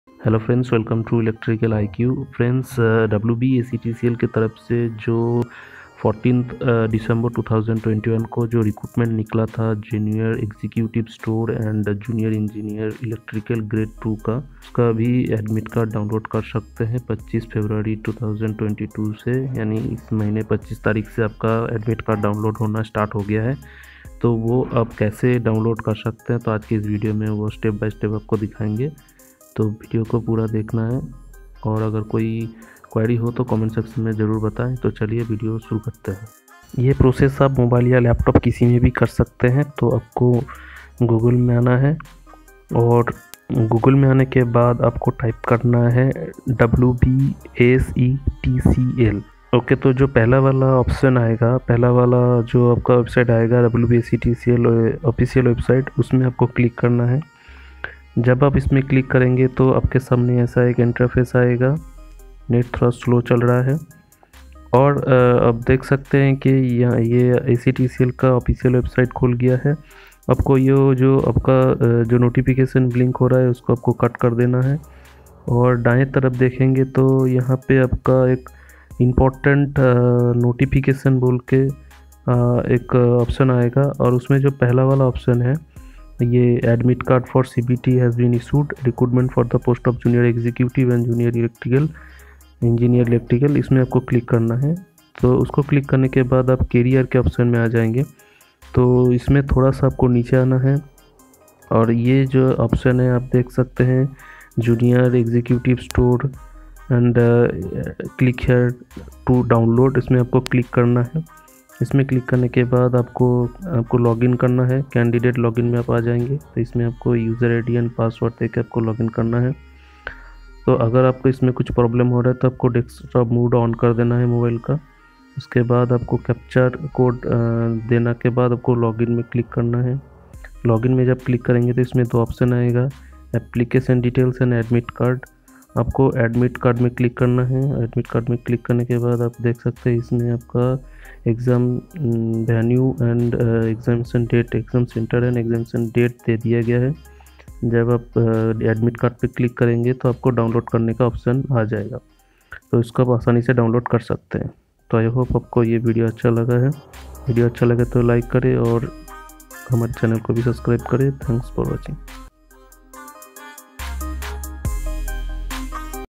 WBSETCL Admit Card Download | Post of Jr. Engineer (Electrical) Gr.-ll & Jr. Executive (Stores) 2022
In this video topic

1. How to download wbsetcl admit Card 2022
2. Junior Engineer Electrical Requirement 2022
3. Admit Card download of WBSETCL Junior Engineer Electrical
4. New Notification of wbsetcl 2022